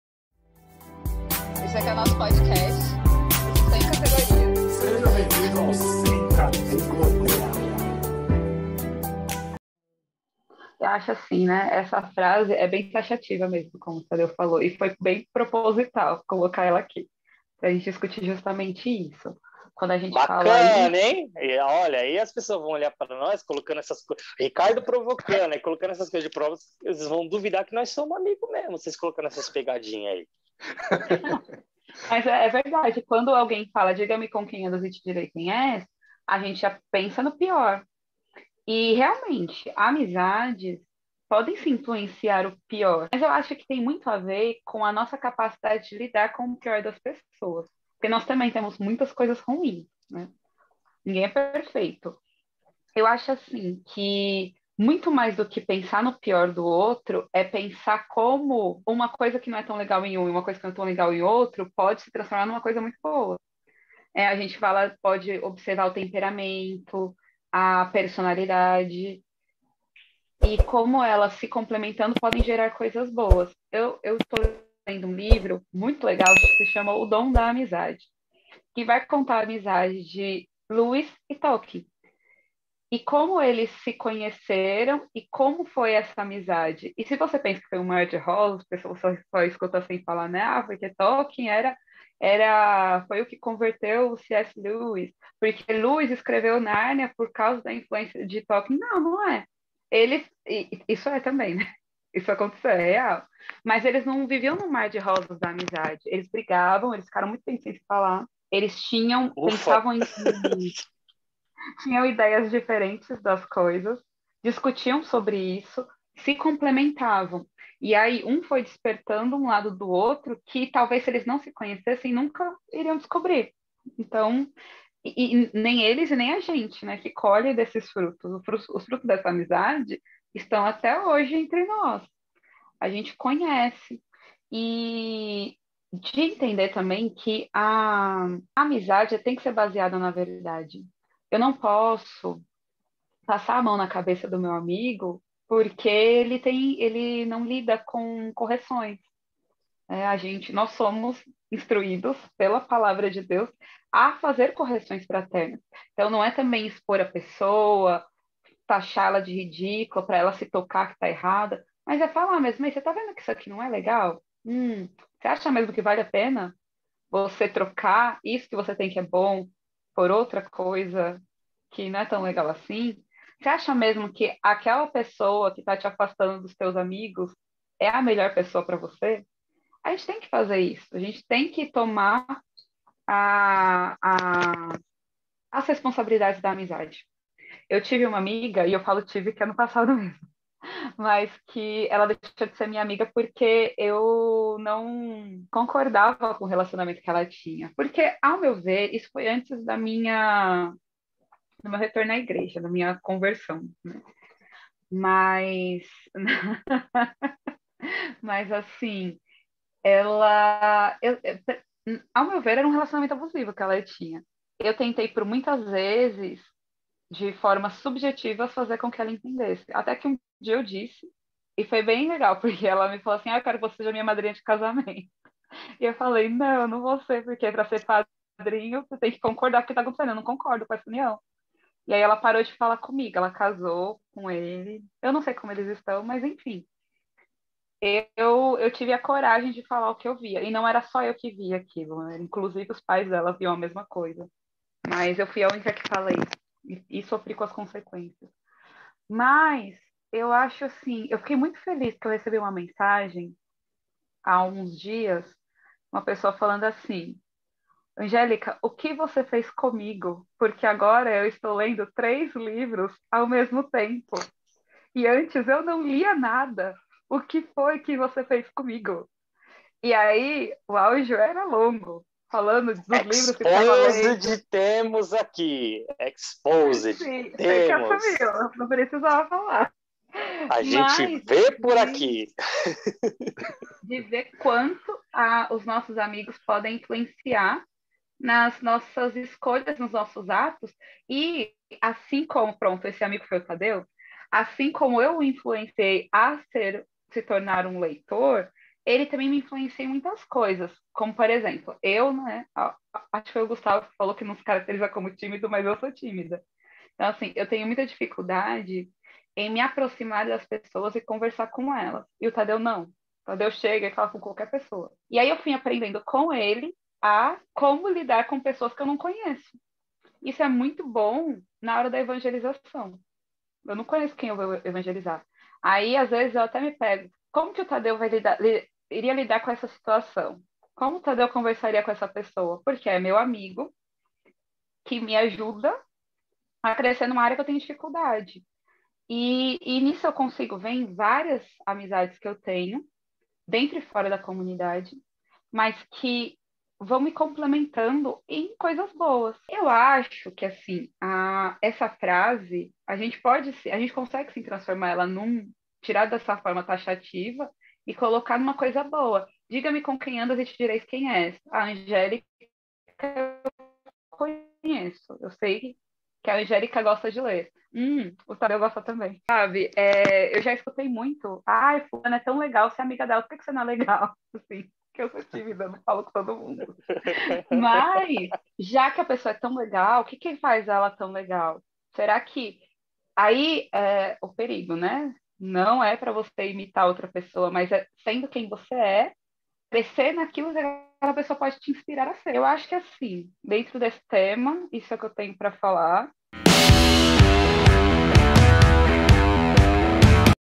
PSC - CORTES DE CATEGORIA - TUDO QUE VOCÊ PRECISA PARA SER UM AMIGO DE VERDADE
BOM DIA, BOA TARDE E BOA NOITE PODCASTS SEM CATEGORIA NO AR, CHEGAMOS!!!

Vamos falar hoje  de tudo que você precisa para ser um bom amigo, um amigo de categoria, nossa reflexão será sobre amizade e como ela faz a diferença no nosso amadurecimento e desenvolvimento pessoal e social, se liga nessa reflexão de categoria e responde nos comentários você é um amigo de verdade??
Ser for deixa aquele like, aproveite o papo SHALOM